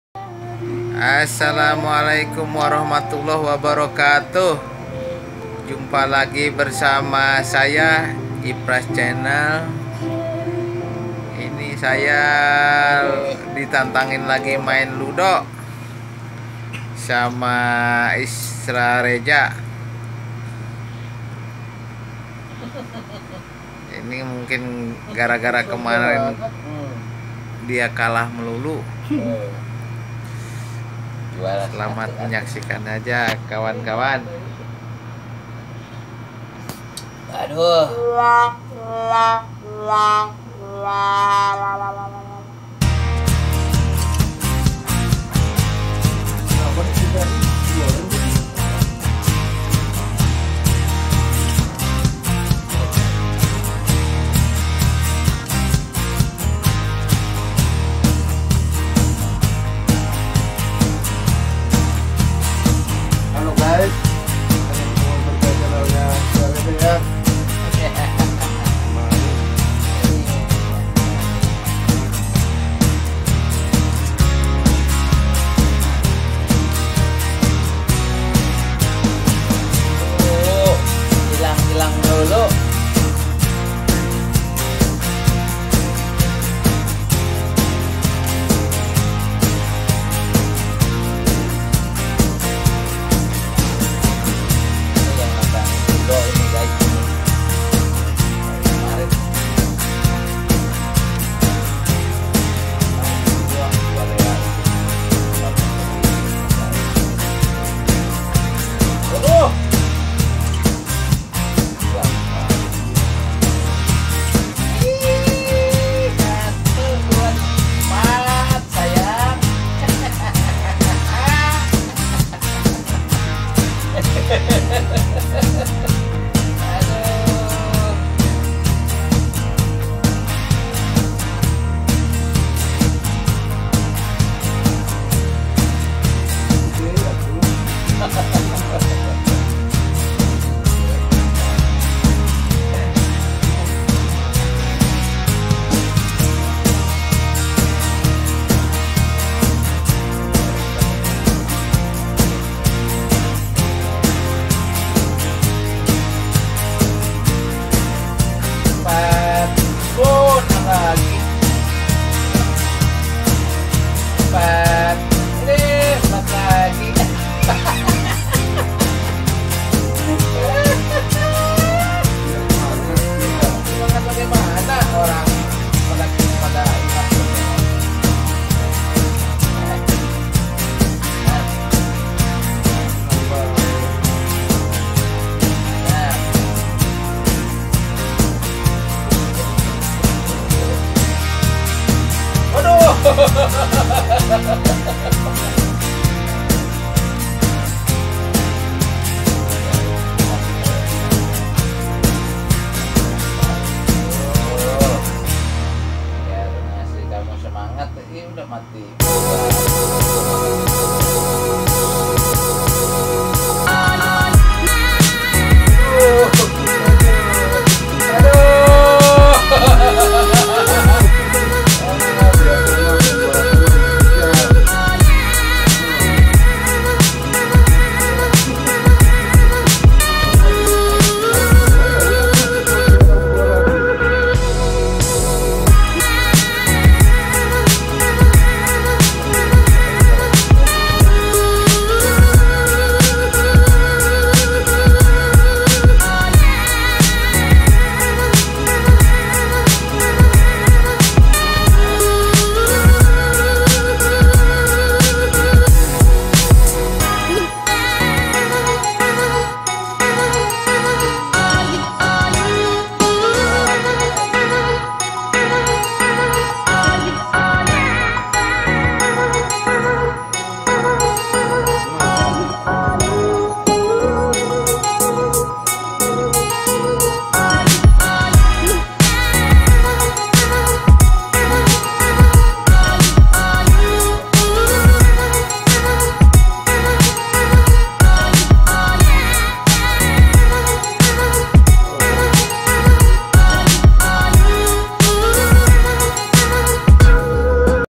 Lagi main ludo sama kawan karena kemarin kalah melulu jadi nantangin lagi nih
Lagi main ludo bersama kawan karena dia kalah melulu jadi nantangin lagi  ...